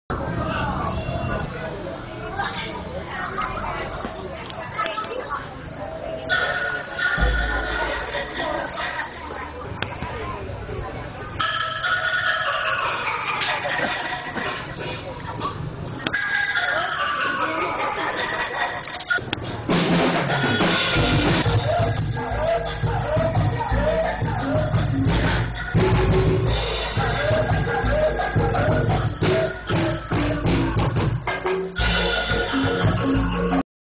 Grosir Sandal - Penjual Mainan -Penjual Pecel dan Pengamen Pasar Beringharjo Yogyakarta
suasan tradisional di Pasar Beringharjo Yogyakarta